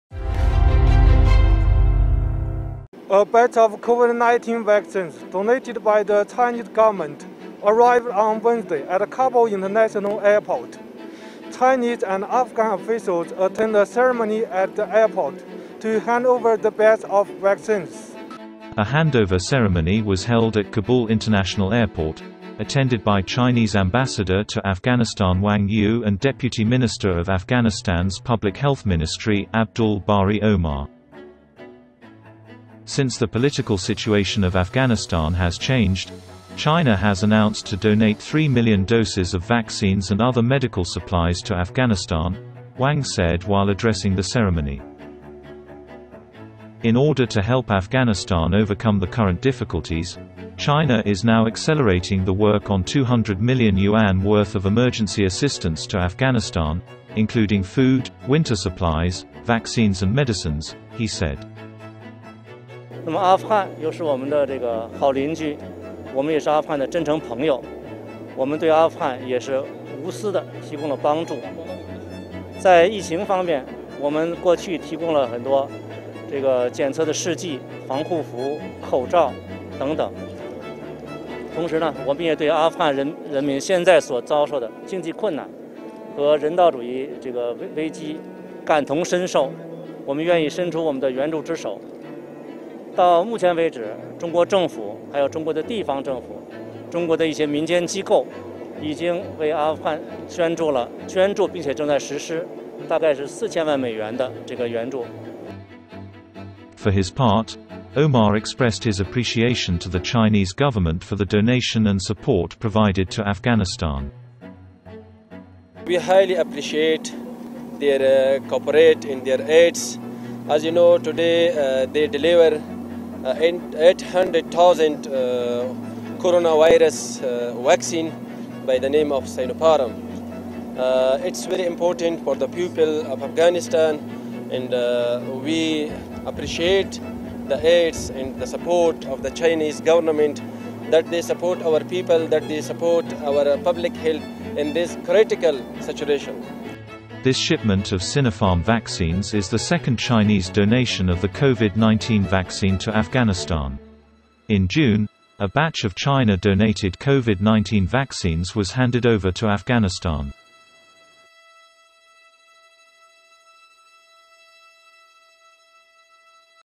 အာဖဂန်နစ္စတန်နိုင်ငံသို့ တရုတ်နိုင်ငံမှပေးပို့လှူဒါန်းသည့် COVID-19ကာကွယ်ဆေး တစ်သုတ်ထပ်မံ ရောက်ရှိ
အာဖဂန်နစ္စတန်နိုင်ငံသို့ တရုတ်နိုင်ငံမှ ပေးပို့လှူဒါန်းသည့် Sinopharm ကာကွယ်ဆေး နောက်တစ်သုတ်ထပ်မံ ရောက်ရှိ
ကဘူးလ်၊ ဒီဇင်ဘာ ၁၀ ရက် (ဆင်ဟွာ)
တရုတ်နိုင်ငံ အစိုးရ က ပေးပို့လှူဒါန်းသည့် COVID-19 ကာကွယ်ဆေး နောက်တစ်သုတ်သည် အာဖဂန်နစ္စတန်နိုင်ငံ ကဘူးလ်မြို့သို့ ဒီဇင်ဘာ ၈ ရက်နေ့က ရောက်ရှိခဲ့သည်။
ကာကွယ်ဆေးများလွှဲပြောင်းပေးအပ်ပွဲအခမ်းအနားကို ကဘူးလ် နိုင်ငံတကာလေဆိပ်တွင်ကျင်းပခဲ့ပြီး အာဖဂန်နစ္စတန်နိုင်ငံဆိုင်ရာ တရုတ်နိုင်ငံ သံအမတ်ကြီး ဝမ်ယွီ နှင့် အာဖဂန်နစ္စတန်နိုင်ငံ ပြည့်သူ့ကျန်းမာရေး ဝန်ကြီးဌာန ဒုတိယဝန်ကြီး အဗ္ဗဒူလ် ဘာရီ အိုမာ (Abdul Bari Omar) တို့တက်ရောက်ခဲ့သည်။
အာဖဂန်နစ္စတန်နိုင်ငံ၌ နိုင်ငံရေးအခြေအနေပြောင်းလဲကတည်းက တရုတ်နိုင်ငံက အာဖဂန်နစ္စတန်နိုင်ငံကို ကာကွယ်ဆေး အလုံးရေ ၃ သန်းနှင့် အခြားဆေးဘက်ဆိုင်ရာအထောက်အပံ့များကို ပံ့ပိုးပေးမည်ဟု ကြေညာထားခဲ့ကြောင်း အခမ်းအနားတွင် သံအမတ်ကြီး ဝမ်က ပြောကြားခဲ့သည်။
အာဖဂန်နစ္စတန်၏ လက်ရှိကြုံတွေ့နေရသည့်အခက်အခဲများအား ကျော်လွှားနိုင်ရန် ကူညီသည့်အနေဖြင့် တရုတ်နိုင်ငံသည် အစားအသောက်များ၊ ဆောင်းရာသီအထောက်အပံ့များ၊ ကာကွယ်ဆေးများနှင့် ဆေးဝါးများ အပါအဝင် ယွမ် သန်း ၂၀၀ (အမေရိကန်ဒေါ်လာ ၃၁ သန်း) တန်ဖိုးရှိသော အရေးပေါ်အထောက်အပံ့များ ကိုလည်း ပေးအပ်သွားရန်လုပ်ဆောင်နေကြောင်းသူကဆိုသည်။
သံအမတ်ကြီးဝမ်က " အာဖဂန်နစ္စတန်နိုင်ငံက အိမ်နီးချင်းကောင်းနိုင်ငံတစ်နိုင်ငံပါ၊  နောက်ပြီး ကျွန်တော်တို့ဟာ အာဖဂန်နစ္စတန်နိုင်ငံနဲ့ မိတ်ဆွေအစစ်အမှန်တွေဖြစ်ကြပါတယ်။ အာဖဂန်နစ္စတန်နိုင်ငံကိုလည်း ကိုယ်ကျိုးမငဲ့ကွက်ဘဲ ကူညီပေးတာပါ။ အရင်တုန်းကလည်း ကျွန်တော်တို့ ရောဂါစမ်းသပ်ကိရိယာတွေ၊ အကာအကွယ်ဝတ်စုံတွေနဲ့ နှာခေါင်းစည်းတွေ စသဖြင့် အများကြီးပေးလှူခဲ့ပြီးပါပြီ။ တစ်ချိန်တည်းမှာပဲ ကျွန်တော်တို့ဟာ အာဖဂန်နိုင်ငံပြည်သူတွေရဲ့ စီးပွားရေးအခက်အခဲနဲ့ လူမှုရေးရာအကြပ်အတည်းတွေအတွက် ကိုယ်ချင်းစာပါတယ်။ ဒါကြောင့်ကျွန်တော်တို့အနေနဲ့ထပ်ပြီးကူညီပေးသွားဖို့ဆန္ဒရှိပါတယ်။ လက်ရှိမှာ တရုတ်နိုင်ငံရဲ့ဗဟိုအစိုးရ ၊ ဒေသန္တရအစိုးရတွေနဲ့ အစိုးရမဟုတ်တဲ့အဖွဲ့အစည်းတွေက အမေရိကန်ဒေါ်လာ သန်း ၄၀ ဝန်းကျင်ရှိတဲ့ အကူအညီအထောက်အပံ့တွေ အာဖဂန်နစ္စတန်နိုင်ငံကို ထောက်ပံ့ကူညီပေးပြီးပါပြီ" ဟု ဆိုသည်။
ဝမ်က “တရုတ်နိုင်ငံသို့ ထင်းရှုးအခွံမားသီးအရေအတွက်များစွာကို တင်သွင်းခွင့်ပြုပြီး အာဖဂန်နစ္စတန်နိုင်ငံရှိ ထင်ရှုးအခွံမာသီးစိုက်ပျိုးသည့် လယ်သမားများ၏ ဝင်ငွေ အမေရိကန်ဒေါ်လာသန်းနှင့်ချီ ရရှိစေရန် အကူအညီပေးနေပါတယ်။ ထို့အပြင် တရုတ်နိုင်ငံအနေဖြင့် အာဖဂန်နစ္စတန်နိုင်ငံအား သွင်းကုန်ထပ်မံတိုးချဲ့ခွင့်ပြုမှာဖြစ်ပြီး ပြည်သူများ၏လူနေမှုဘဝ တိုးတက်စေဖို့ ကူညီပေးသွားမှာ ဖြစ်ပါတယ်” မှတ်ချက်ပြုခဲ့သည်။
အာဖဂန်နစ္စတန်နိုင်ငံ ပြည့်သူ့ကျန်းမာရေး ဝန်ကြီးဌာန ဒုတိယဝန်ကြီး အိုမာကလည်း တရုတ်နိုင်ငံအစိုးရ၏ အာဖဂန်နစ္စတန်အား လှူဒါန်းမှုများနှင့်ကူညီမှုများအပေါ် ကျေးဇူးတင်ရှိပါကြောင်း ပြောကြားခဲ့သည်။ ယခု ကဲ့သို့ဆိုးဝါးသည့်အခြေအနေ၌ လှူဒါန်းပေးပို့မှုသည် အာဖဂန်နစ္စတန်နိုင်ငံပြည်သူများ အတွက်အလွန်အရေးကြီးကြောင်း ဆိုသည်။
အာဖဂန်နစ္စတန်နိုင်ငံ ပြည်သူ့ကျန်းမာရေးဝန်ကြီးဌာန ဒုတိယဝန်ကြီး အဗ္ဗဒူ ဘာရီအိုမာက"ကျွန်တော်တို့ ဒီပူးပေါင်းဆောင်ရွက်မှုနဲ့ အကူအညီတွေကိုအထူးကျေးဇူးတင်အသိအမှတ်ပြုပါတယ် ၊ ခင်ဗျားတို့သိတဲ့အတိုင်းပါပဲ။ ဒီကနေ့ Sinopharm တံဆိပ် ကိုရိုနာဗိုင်းရပ်စ်ကာကွယ်ဆေး အလုံးရေ ၈ သိန်းရောက်တာဟာ အာဖဂန်နစ္စတန်နိုင်ငံပြည်သူတွေအတွက် သိပ်ပြီး အရေးကြီးပါတယ်။ နောက်ပြီး ကျွန်တော်တို့ ဟာ တရုတ်နိုင်ငံအစိုးရရဲ့ အကူအညီတွေနဲ့ အထောက်အပံ့တွေကို တန်ဖိုးထားပါတယ်။
သူတို့ဟာ ကျွန်တော်တို့ပြည်သူတွေကို ထောက်ပံ့ပေးပါတယ်၊ ဒီ ခက်ခဲတဲ့အခြေအနေမှာ ကျွန်တော်တို့ပြည်သူတွေရဲ့ ကျန်းမာရေးကိုလည်းထောက်ပံ့ပေးတာဖြစ်ပါတယ်” ဟုဆိုသည်။
ယခုအကြိမ် Sinopharm ကာကွယ်ဆေး တင်ပို့မှုသည် တရုတ်နိုင်ငံမ ှအာဖဂန်နစ္စတန်နိုင်ငံသို့ COVID-19 ကာကွယ်ဆေး ဒုတိယအကြိမ်လှူဒါန်းပေးပို့ခြင်းဖြစ်သည်။ ပြီးခဲ့သည့် ဇွန်လ၌ တရုတ်နိုင်ငံက လှူဒါန်းသည့် COVID-19 ကာကွယ်ဆေးများကို အာဖဂန်နစ္စတန်နိုင်ငံသို့ ပထမဆုံးအကြိမ် ပေးပို့ ခဲ့ကြောင်းသိရသည်။ (Xinhua)
(English Version)
GLOBALink | Afghanistan receives new batch of China-donated Sinopharm vaccines
KABUL, Dec. 9 (Xinhua) -- A batch of COVID-19 vaccines donated by the Chinese government arrived in Kabul, the capital of Afghanistan, on Wednesday.
A handover ceremony was held at Kabul international airport, attended by Chinese Ambassador to Afghanistan Wang Yu and deputy minister of Afghanistan's Public Health Ministry Abdul Bari Omar.
Since the political situation of Afghanistan has changed, China has announced to donate 3 million doses of vaccines and other medical supplies to Afghanistan, Wang said while addressing the ceremony.
In order to help Afghanistan overcome the current difficulties, China is now accelerating the work on 200 million yuan (31 million USD) worth of emergency assistance to Afghanistan, including food, winter supplies, vaccines and medicines, he said.
For his part, Omar expressed his appreciation to the Chinese government for the donation and support provided to Afghanistan. He said the donation is very important for the people of Afghanistan in this critical situation.
This shipment of Sinopharm vaccines is the second Chinese donation of the COVID-19 vaccine to Afghanistan. In June, a batch of China-donated COVID-19 vaccines was handed over to Afghanistan.